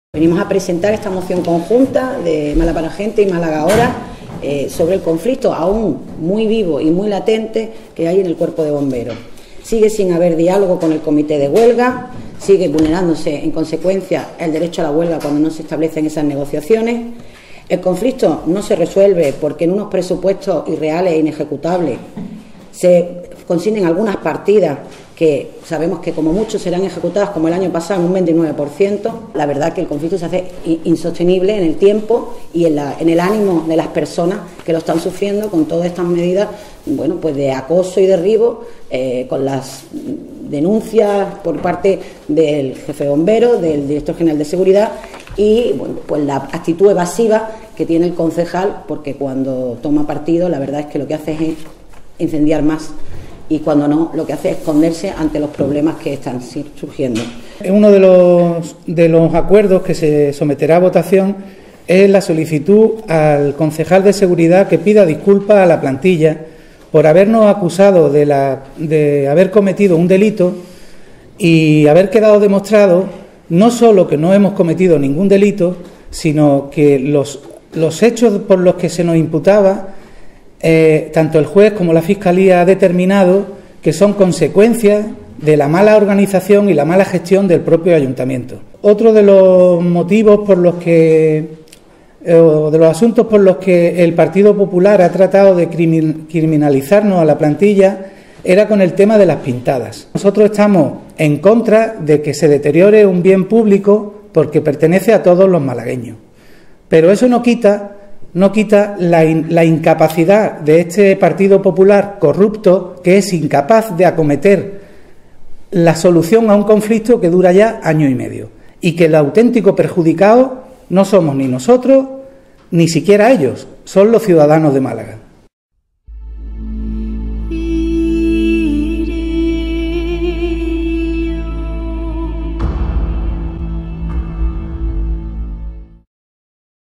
Los bomberos quieren que el concejal de Seguridad se disculpe por criminalizarlos
Moción conjunta de Málaga Ahora y Málaga para la Gente tras las últimas sentencias absolutorias a lxs bomberxs.